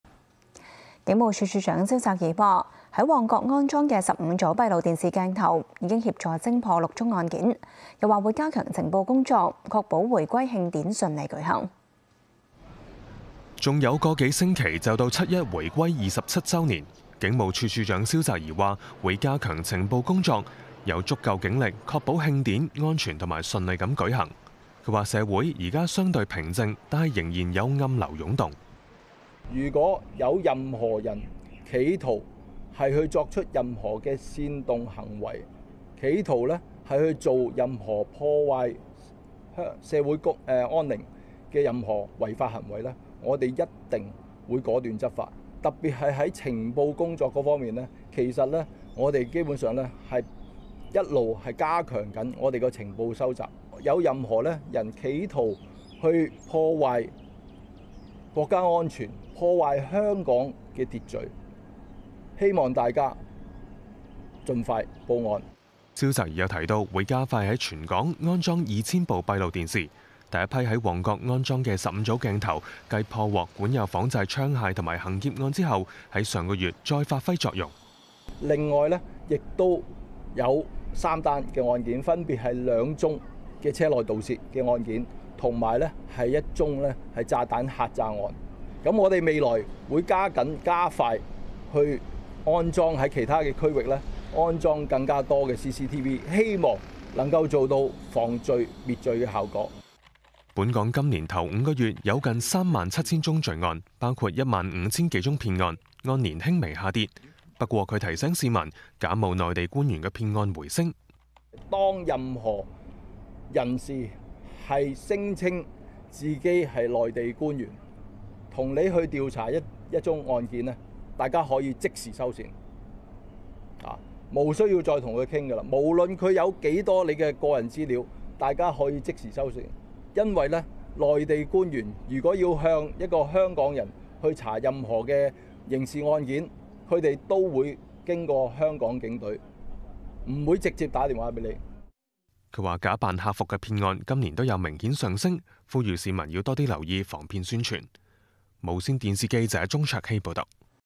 香港無綫｜香港新聞｜22/06/2024 要聞｜蕭澤頤指加快安裝全港2000部閉路電視 首批旺角15組鏡頭助破六宗案件
警務處處長蕭澤頤指，在旺角安裝的15組閉路電視鏡頭，已協助偵破六宗案件。又指會加強情報工作，確保回歸慶典順利舉行。

還有個多星期，就到七一回歸27周年。警務處處長蕭澤頤指，會加強情報工作，有足夠警力確保慶典，安全、順利舉行。他指社會現時相對平靜，但仍有暗流湧動。

警務處處長蕭澤頤說：「如果有任何人企圖作出任何煽動行為，企圖破壞社會安寧，任何違法行為，我們一定會果斷執法。特別在情報工作上，我們一直在加強情報收集。任何人企圖破壞，國家安全、香港秩序，希望大家盡快報案。」

蕭澤頤又提到，會加快在全港安裝2,000部閉路電視，首批在旺角安裝的15組鏡頭，繼破獲管有仿製槍械及行劫案後，上月再發揮作用。

蕭澤頤稱：「另外有三宗案件，分別兩宗車內盜竊案，及一宗炸彈嚇詐案，我們未來會加緊、加快，在其他區域安裝更多閉路電視，希望能做到防罪滅罪效果。」

本港今年首5個月，有近3.7萬宗罪案，包括1.5萬多宗騙案。按年輕微下跌，但提醒市民，假冒內地官員騙案回升。

蕭澤頤表示：「當任何人聲稱，自己是內地官員，與你調查一宗案件，大家可以即時收線，毋須再與他談。無論他有多少你的個人資料，都可以即時收線。因為內地官員，如果要向港人調查任何刑事案件，他們都會經過香港警隊，不會直接致電給你。」

他指假扮客服騙案，今年都有明顯上升，呼籲市民多留意防騙宣傳。